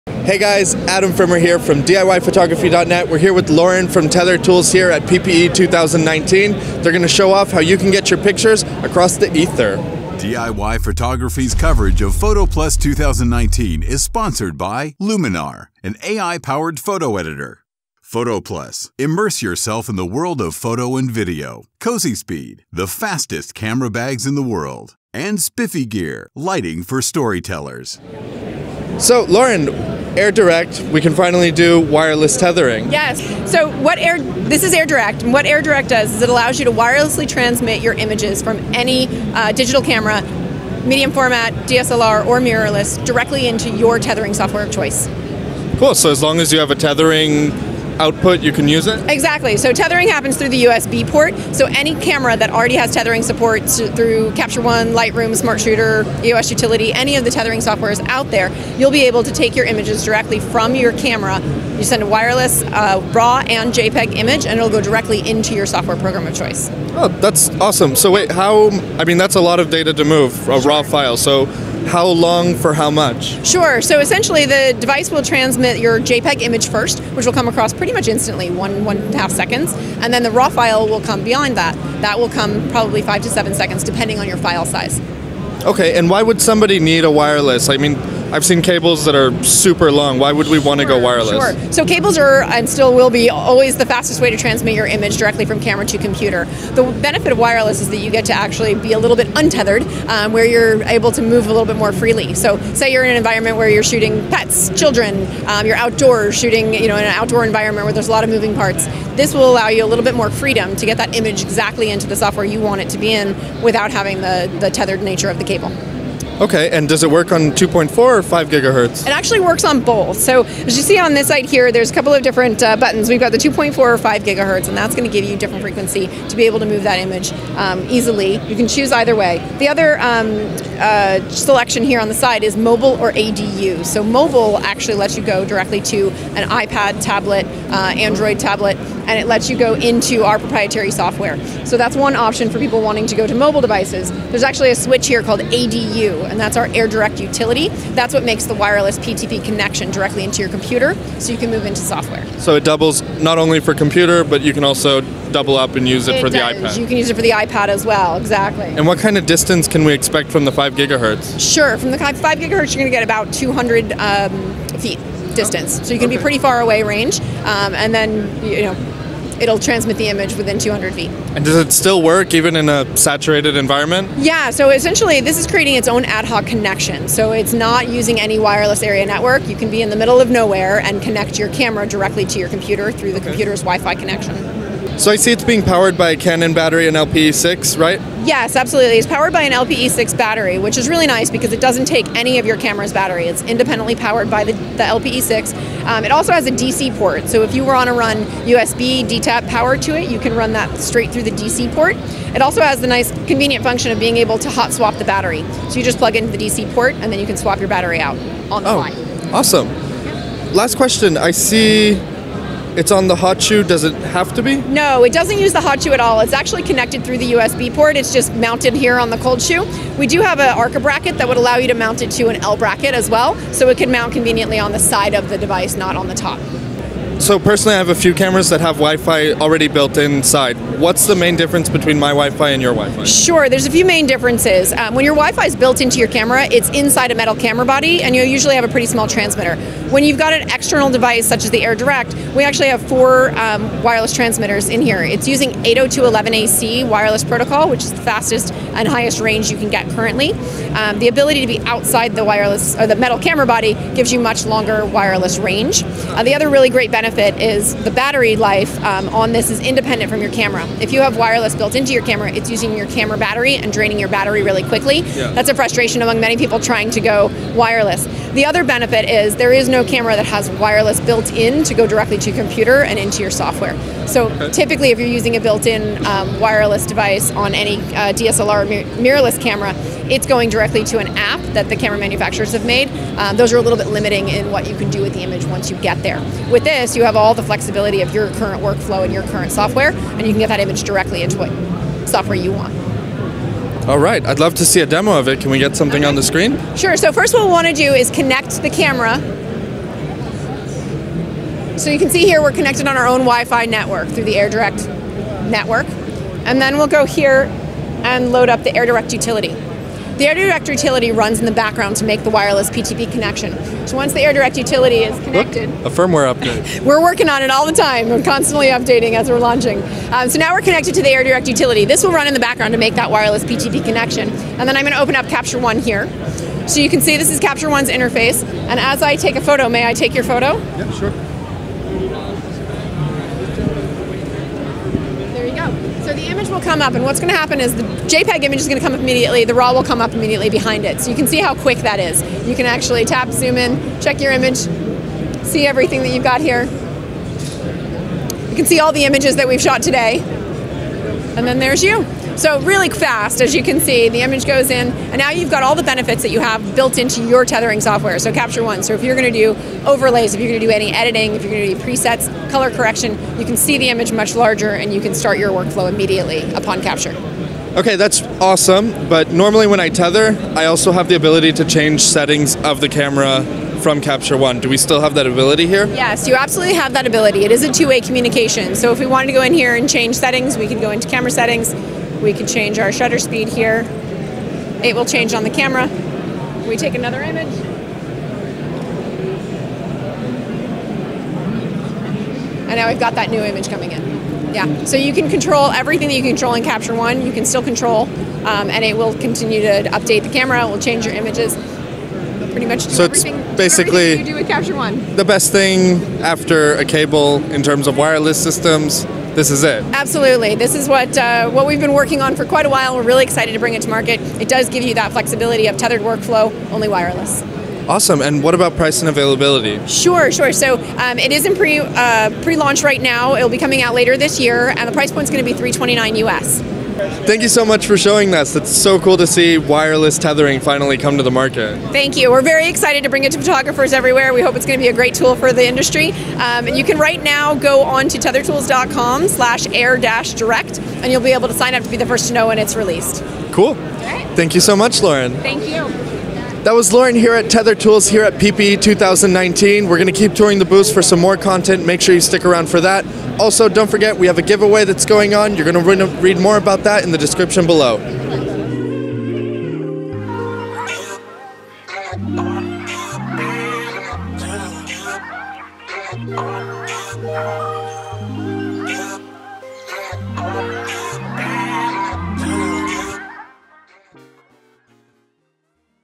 Wirelessly tether any camera to your desktop with Air Direct
It’s been three years since we saw the Tether Tools Case Air unveiled at Photokina 2016. It offered a lot of options for wirelessly tethering your camera to their dedicated app running on a tablet. Now, though, the new Tether Tools Air Direct lets you wirelessly tether any camera using your regular desktop software. Lightroom, Capture One, or whatever you want.

We caught up with Tether Tools at PhotoPlus 2019 to find out more about it and its capabilities.